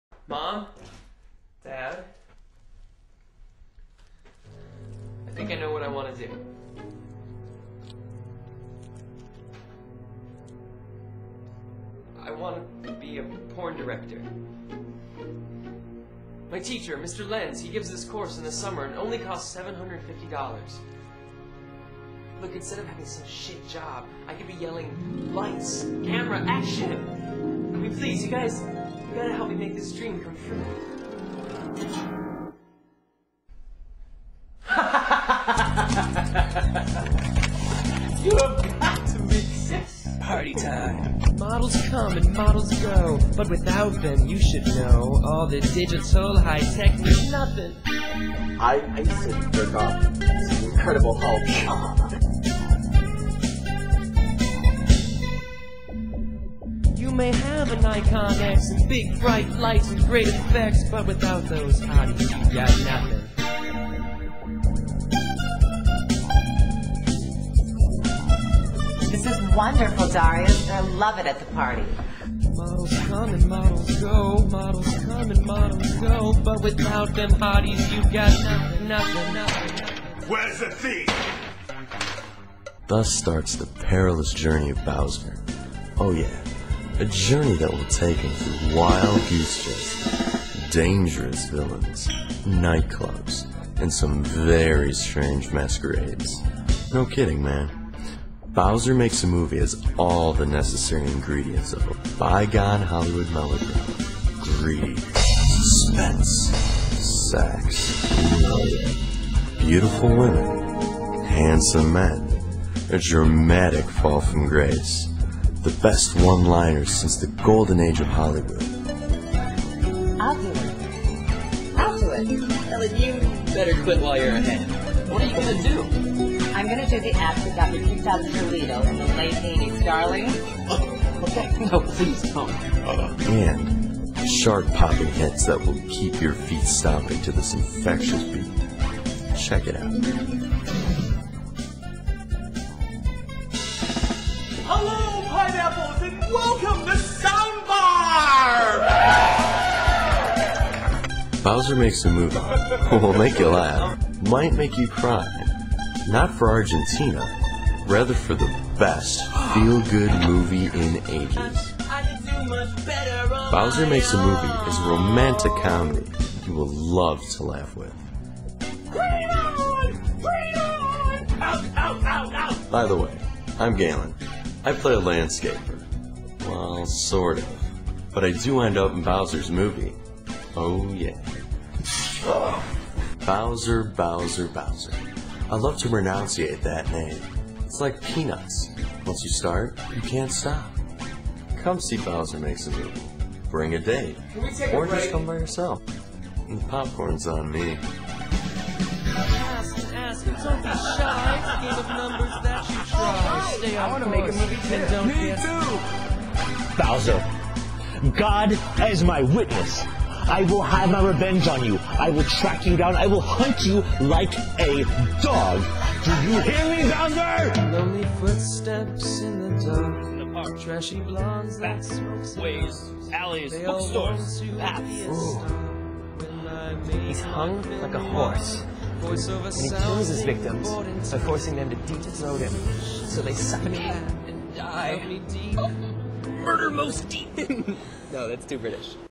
Bowser Makes a Movie
A dream of being a Porn Director goes terribly wrong for a youth who with his best friend try to get the dough for this t venture.